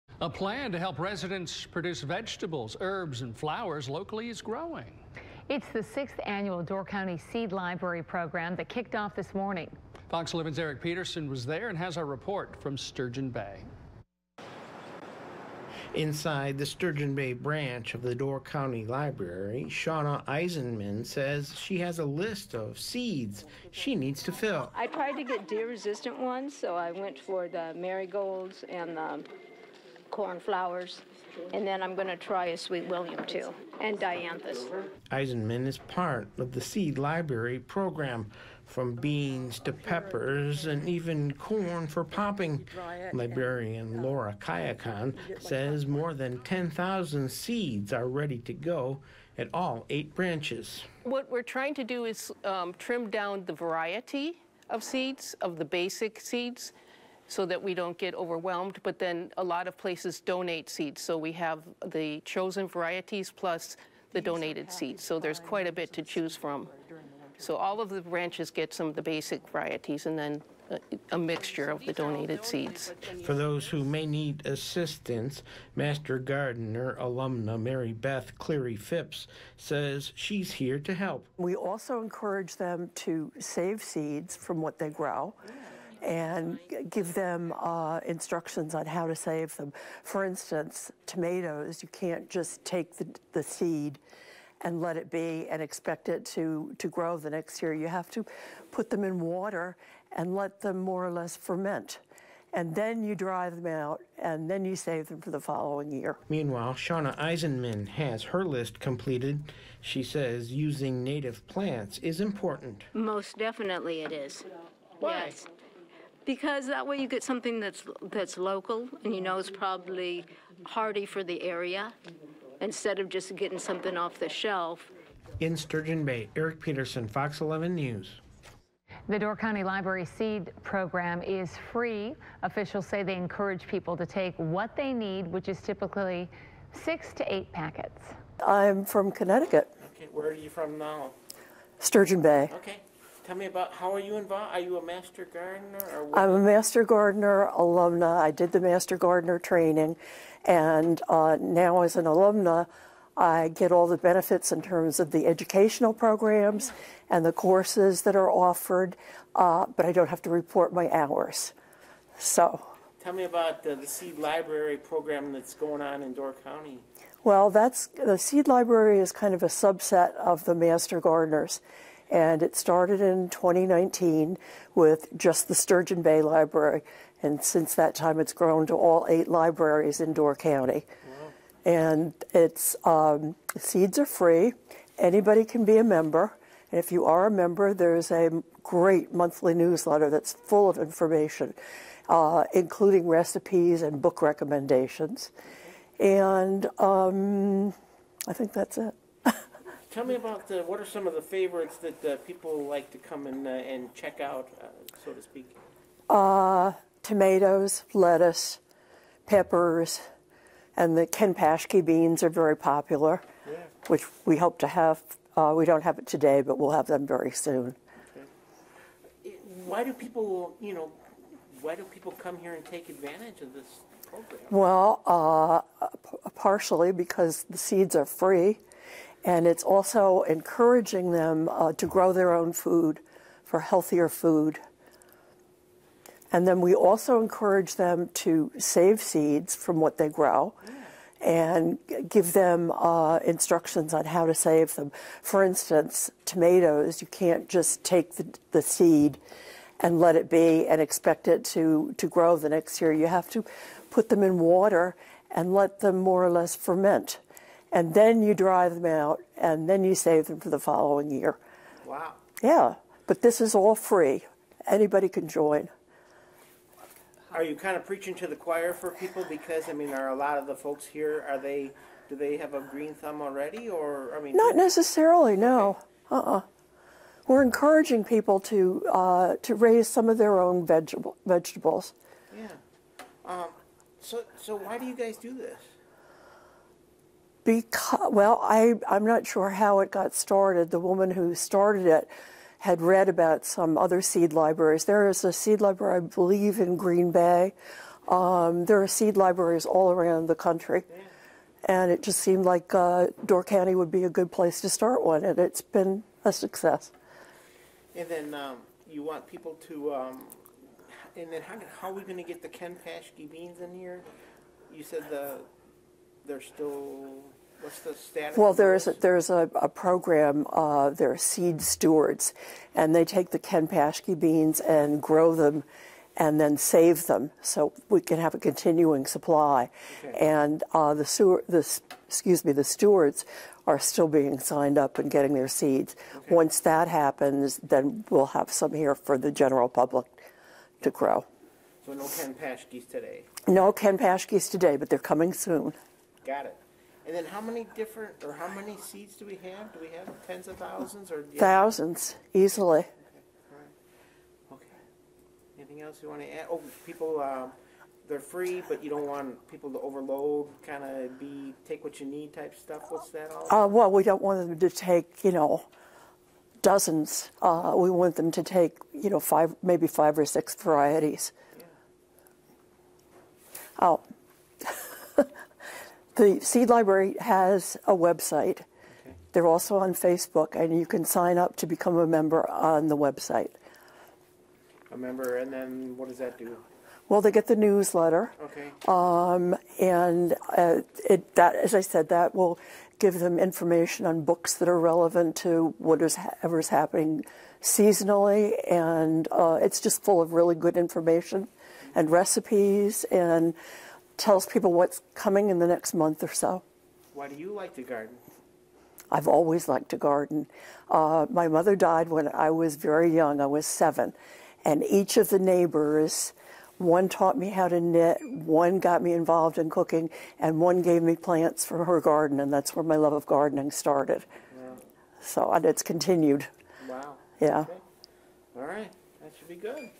Door County Seed Library inspires gardeners
A plan to help Door County residents produce vegetables, herbs and flowers locally is growing.